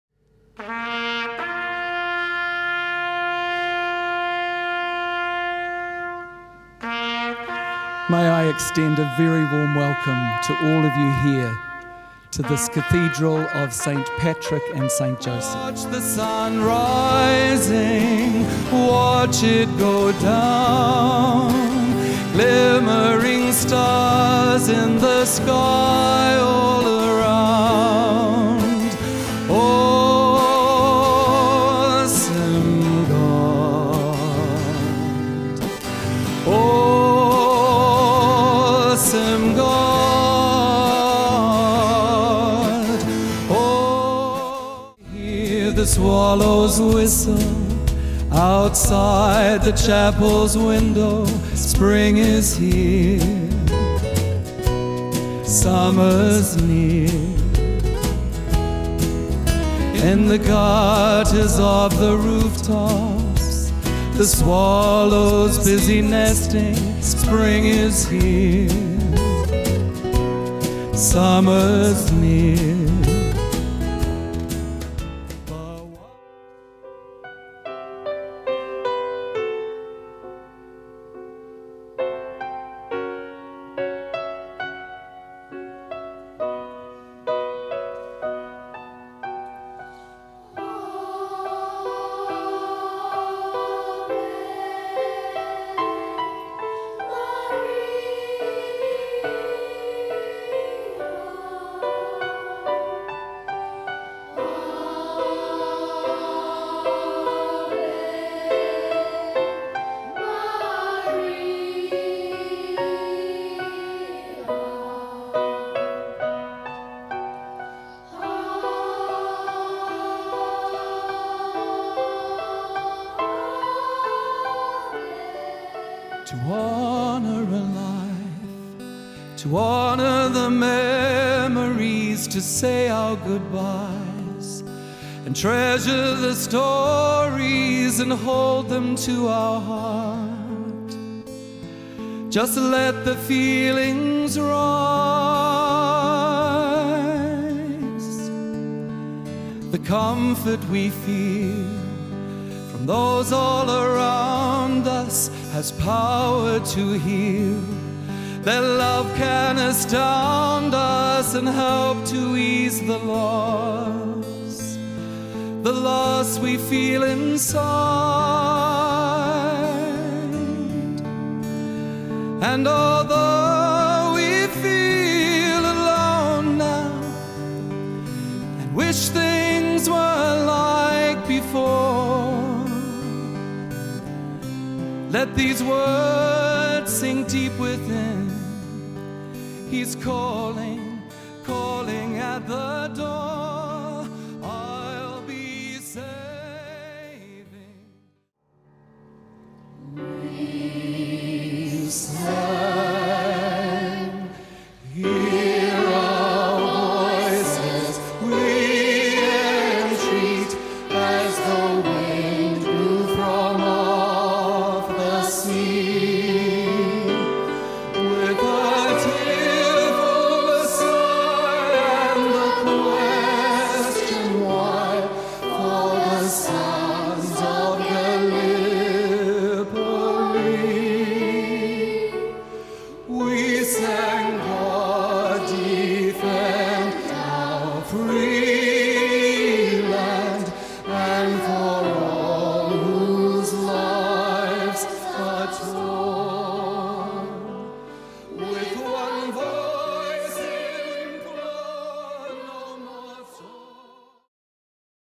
FrChrisSkinnerSM - St Patricks DVD Trailer - 21 May 2011
Fr. Chris Skinner SM performing live at St Patricks Cathedral, Auckland, 21 May 2011 featuring The Auckland Boys Choir.  Musical Director, Stuart Weightman. Pianist, Edwin Randell. To purchase a DVD or further information